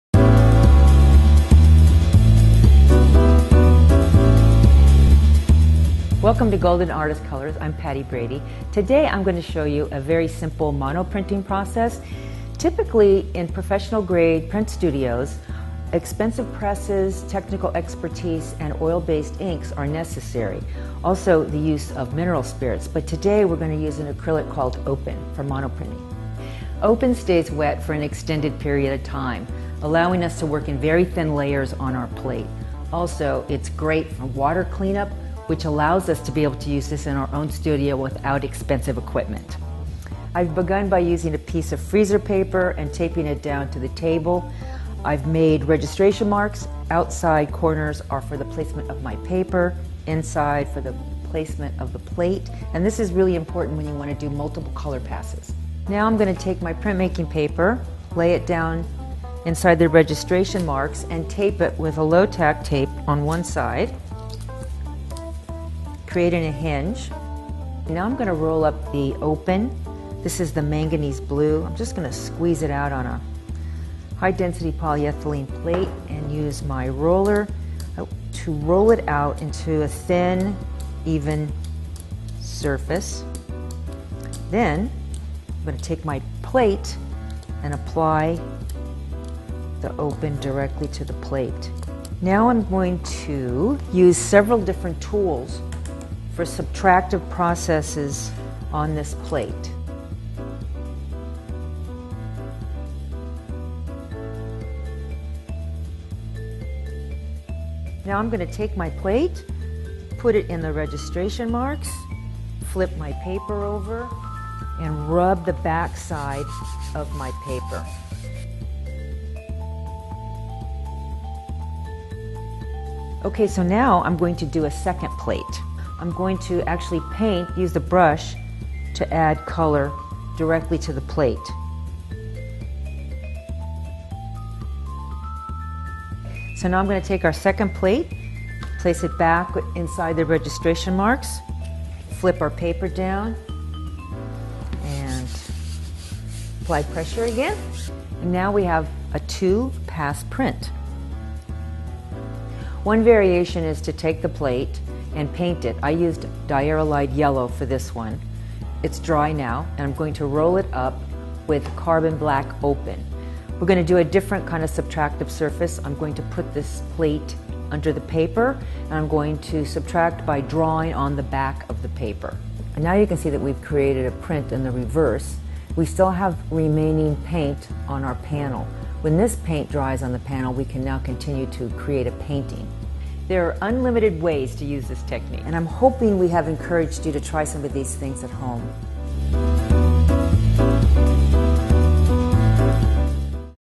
Making Monoprints with Golden Open Acrylic Colour
Monoprinting with Golden Open Acrylic Colour - how to use Golden slow drying Open Acrylic to create simple prints without the need for expensive presses, inks or solvents.

Golden Open Acrylics are a truly unique acrylic paint range.  They are formulated with a relaxed set of working characteristics which give them the feature of having a prolonged drying time.  This means that they are workable for a similar length of time as a professional Artist oil colour, with the added bonus of one not having to use very abrasive and harmful solvents when working. So you can blend, soften, shade, glaze and use fine detail with your open acrylic colours without any danger of the paint drying too quickly.